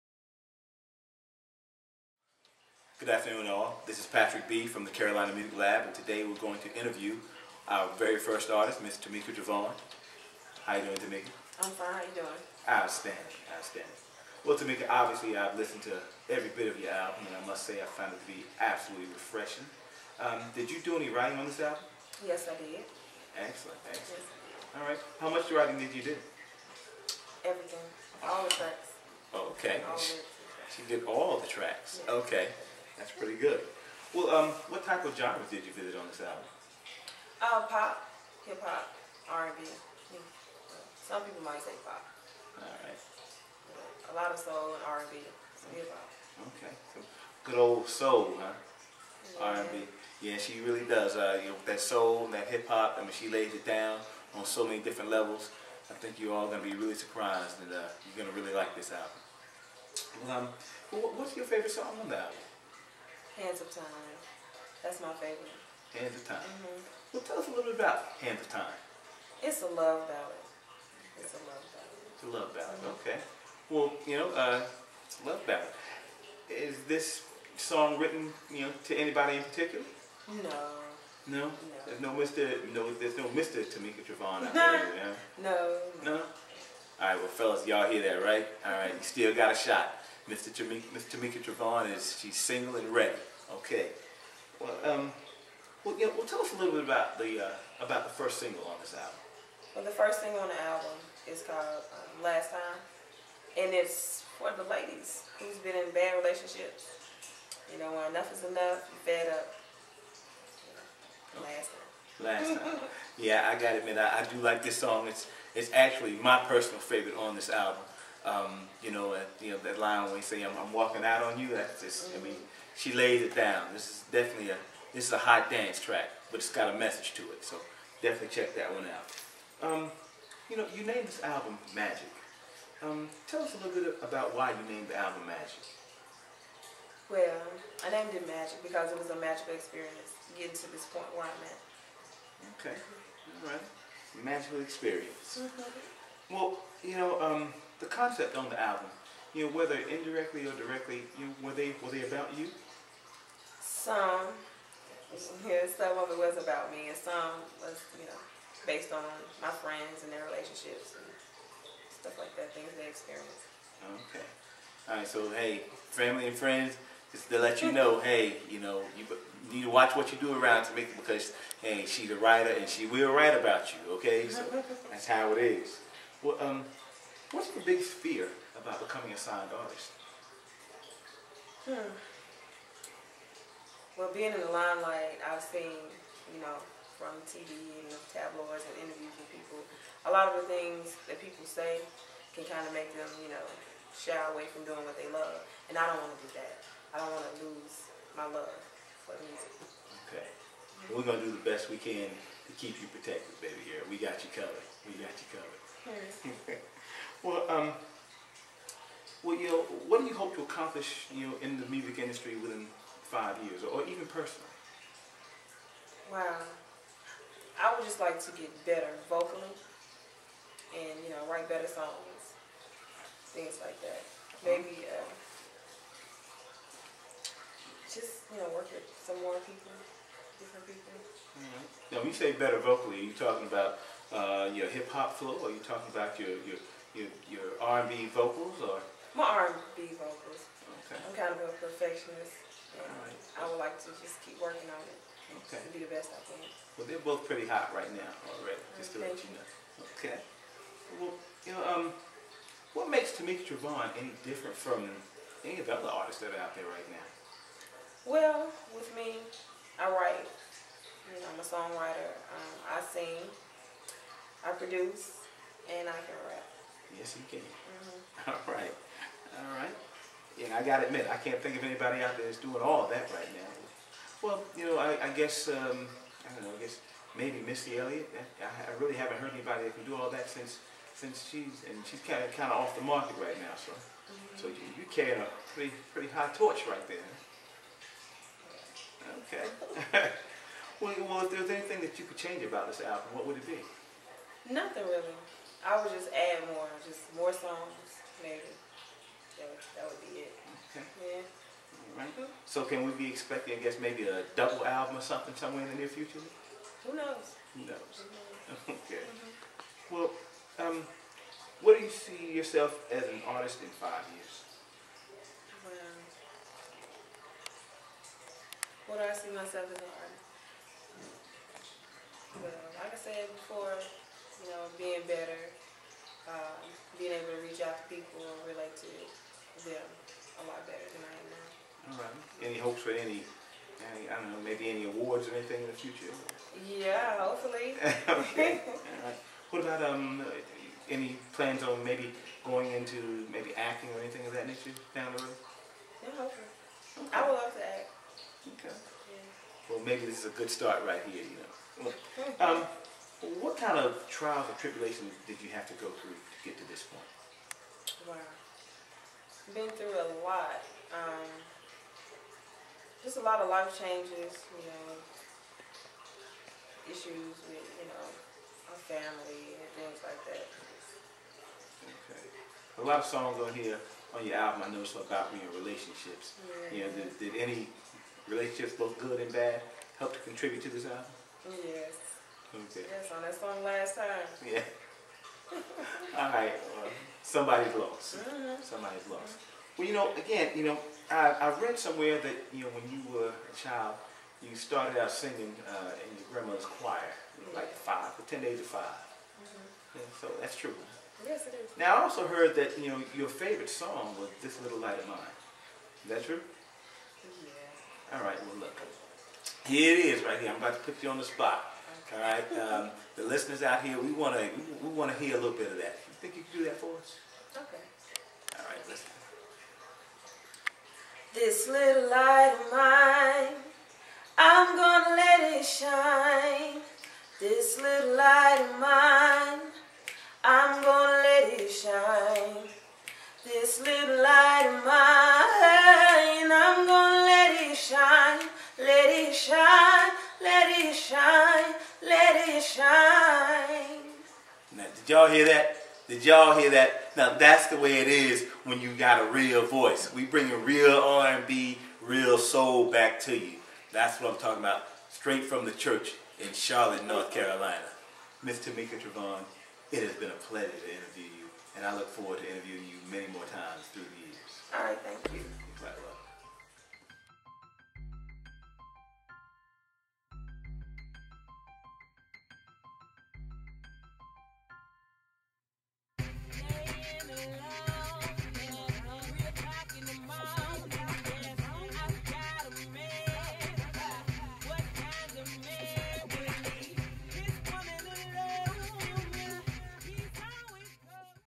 Tameka Travon In-Depth Interview
A short in-depth interview of Carolina Music Lab recording artist Tameka Travon. Find out what makes Tameka Travon click and more.